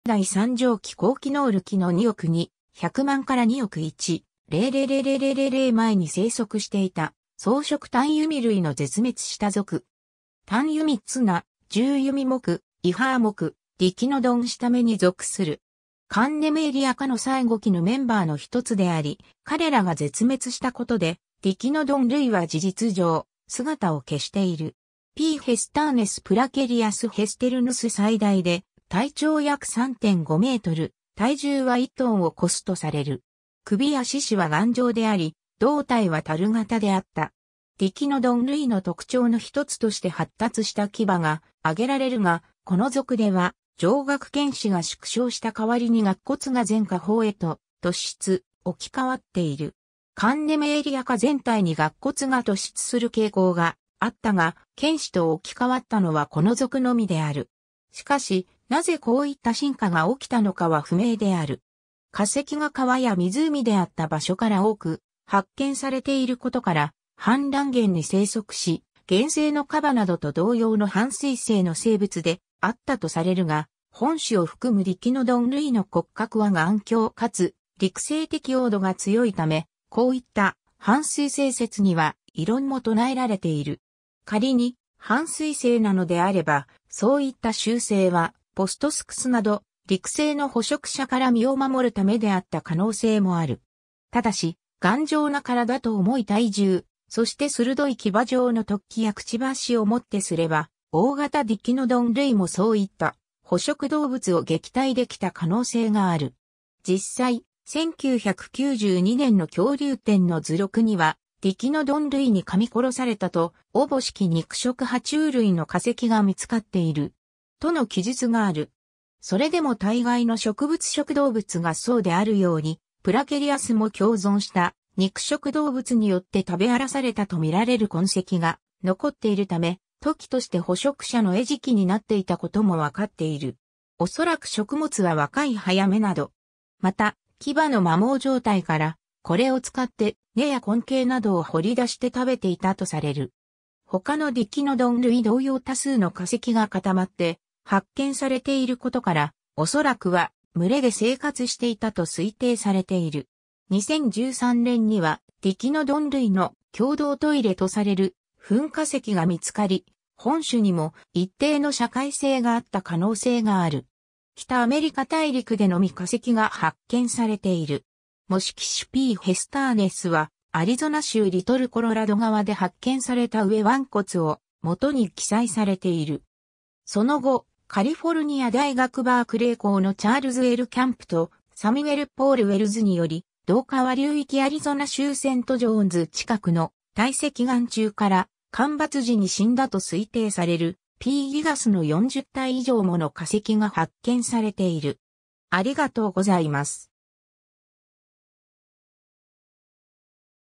プラケリアス
プラケリアス (Placerias) は、中生代三畳紀後期ノール期の2億2,100万 - 2億1,000万年前に生息していた草食単弓類（旧「哺乳類型爬虫類」）の絶滅した属。
単弓綱・獣弓目・異歯亜目・ディキノドン下目に属する。
カンネメエリア科の最後期のメンバーの一つであり、かれらが絶滅したことでディキノドン類は事実上、姿を消している。
 P. hesternus プラケリアス・ヘステルヌス 最大で体長約3.5m、体重は1tを超すとされる。
首や四肢は頑丈であり、胴体は樽型であった。
ディキノドン類の特徴の一つとして発達した牙が挙げられるが、この属では上顎犬歯が縮小した代わりに顎骨が前下方へと突出、置き換わっている。
カンネメエリア科全体に顎骨が突出する傾向があったが、犬歯と置き換わったのはこの属のみである。
しかし、何故こういった進化が起きたのかは不明である。
 化石が川や湖であった場所から多く発見されていることから、氾濫原に生息し、現生のカバなどと同様の半水生の生物であったとされるが、本種を含むディキノドン類の骨格は頑強かつ陸棲適応度が強いため、こういった半水棲説には異論も唱えられている。
仮に半水棲なのであれば、そういった習性はポストスクスなど陸生の捕食者から身を守るためであった可能性もある。
ただし、頑丈な身体と重い体重、そして鋭い牙状の突起や嘴をもってすれば、大型ディキノドン類もそういった捕食動物を撃退できた可能性がある。
実際、1992年の恐竜展の図録には、「ディキノドン類に噛み殺されたと思しき肉食爬虫類の化石が見つかっている。」との記述がある。
 それでも大概の植物食動物がそうであるように、プラケリアスも共存した肉食動物によって食べ荒らされたとみられる痕跡が残っているため、時として捕食者の餌食になっていた事も分かっている。
 おそらく食物は若い葉や芽など。
また、牙の摩耗状態から、これを使って根や根茎などを掘り出して食べていたとされる。
他のディキノドン類同様多数の化石が固まって発見されている事から、おそらくは群れで生活していたと推定されている。
2013年にはディキノドン類の“共同トイレ”とされる糞化石が見つかり、本種にも一定の社会性があった可能性がある。
 北アメリカ大陸でのみ化石が発見されている。
模式種 P. hesternus はアリゾナ州リトルコロラド川で発見された上腕骨を元に記載されている。
その後、カリフォルニア大学バークレー校のチャールズ・L・キャンプ (Charles_Lewis_Camp) とサミュエル・ポール・ウェルズ (Samuel_Paul_Welles) により、同川流域アリゾナ州セント・ジョーンズ近くの堆積岩中から、旱魃時に死んだと推定されるP・ギガス (P. gigas) の40体以上もの化石が発見されている。